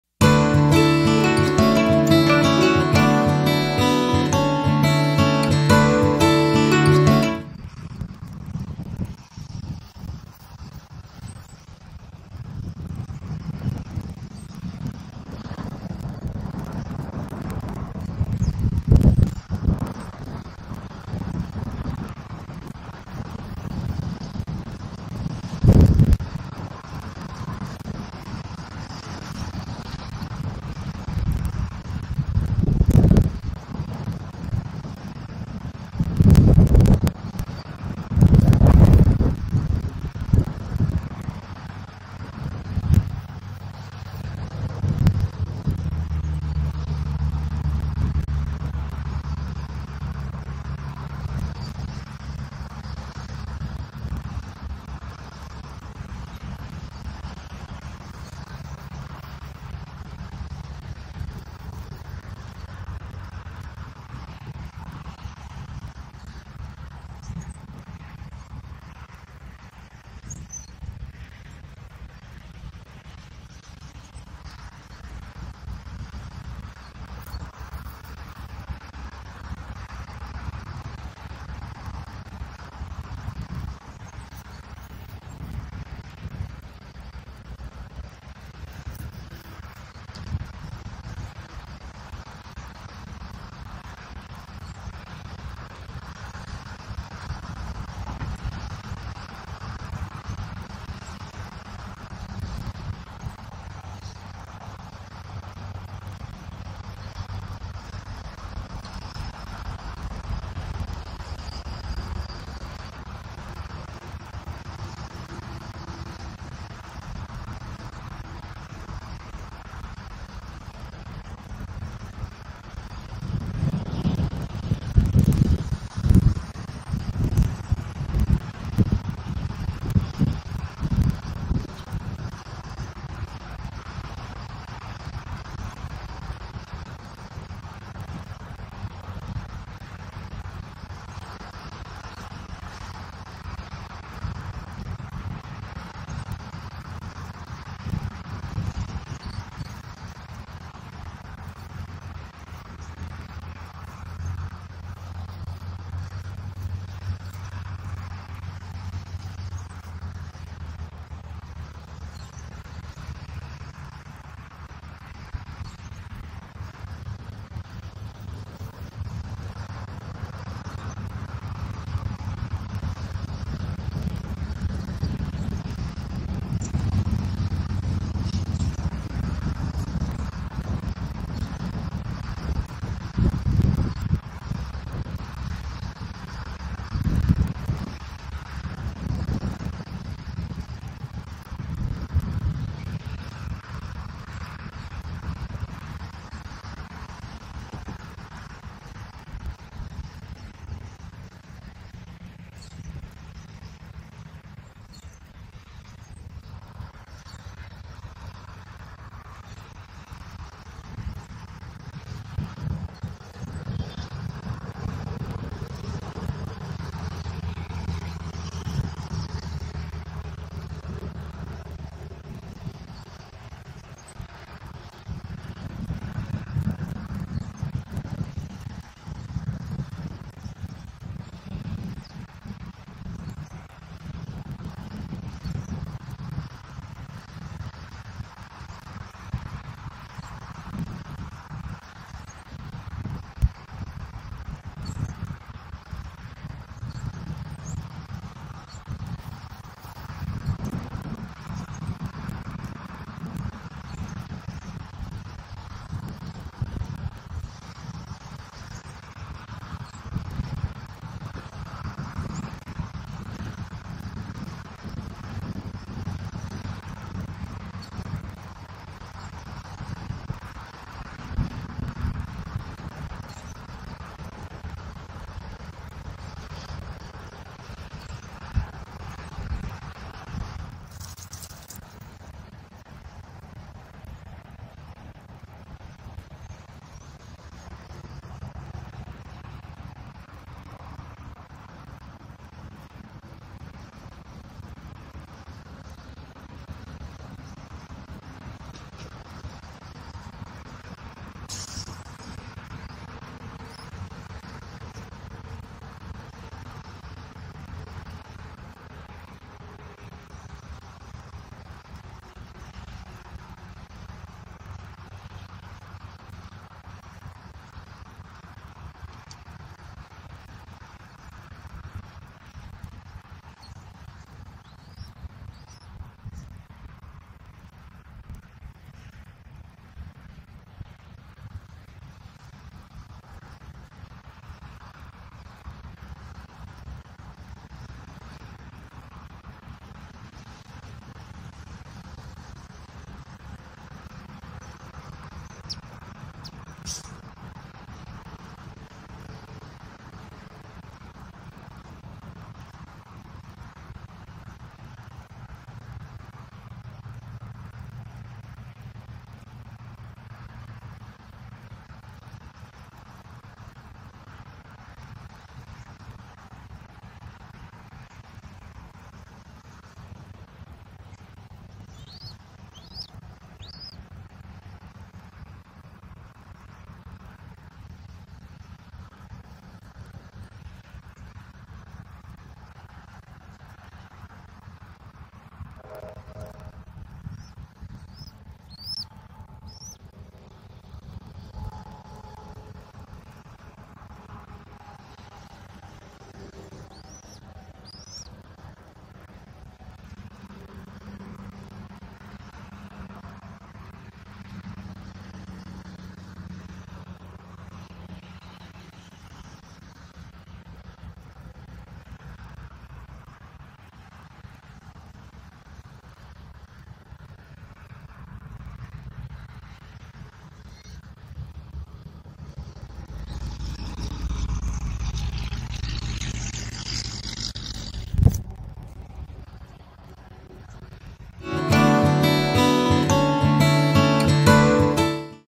Bath Time For Dad & What An Eaglet Does When Home Alone - SWFL Nest of M15, F23 & E23 (1/3/24)
In case you were wondering why so many celebrated when the SWFL pond cam went live - M15's baths are surely part of the reason. 😂 See for yourself!

And, if you want to know how a little eaglet spends a few minutes "home alone"... s/he practices turning around in a full circle in the nest!

Sometimes, it's so refreshing to see the cute stuff, am I right?

E23 hatched 12/31/23 at 7:07am

Screen rec from Southwest Florida Eagle Cam (Jan. 3, 2024)